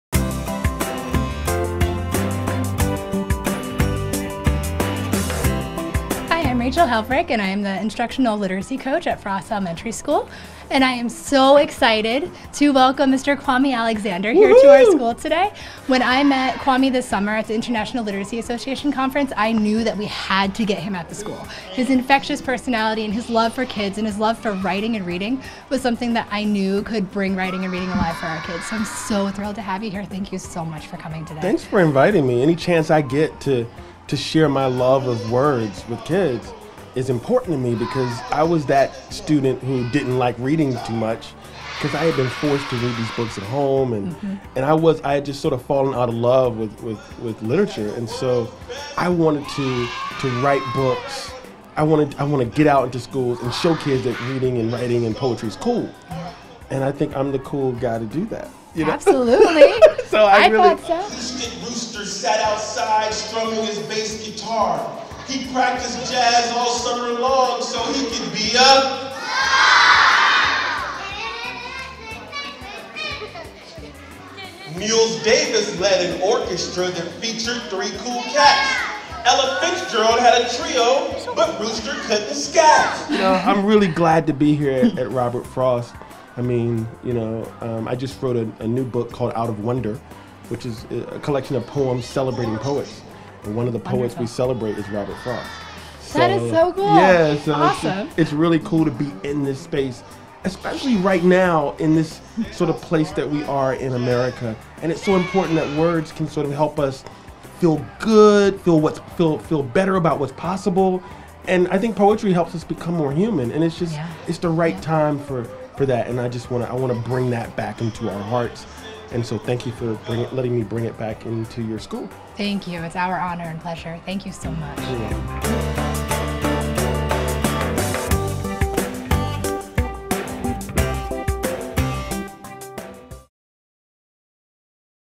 Kwame Alexander at Frost School
Author Kwame Alexander read selections from his books to students at the Frost Elementary School.